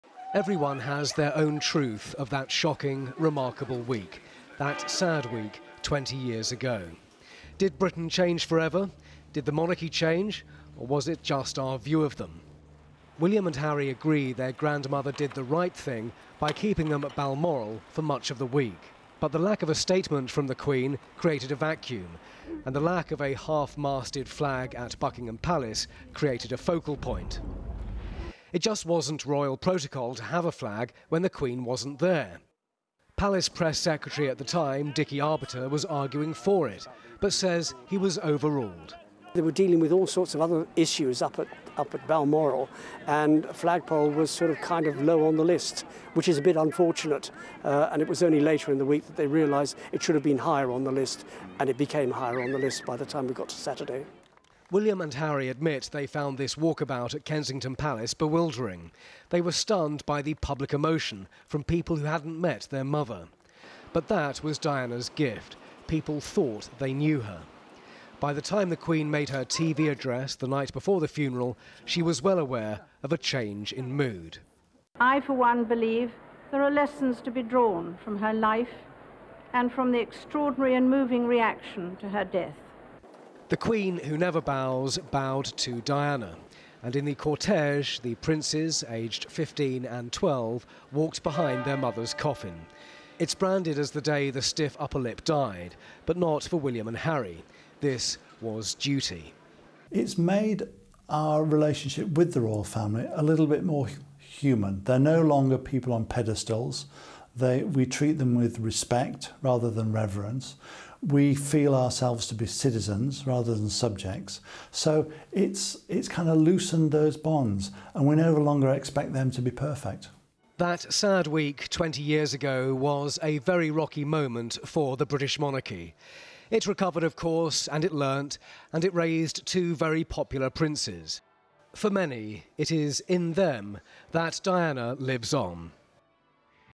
Diana 20th Anniversary: Her legacy lives on
Diana's death sparked a nationwide mourning, unlike any seen before or since. For her sons, William and Harry, their private grief was made into a very public affair. Yet the apparently muted reaction of the wider Royal Family caused anger among many - and its effects have had repercussions to this day.

This special report looks at the impact of Diana's death on Britain and the Royals.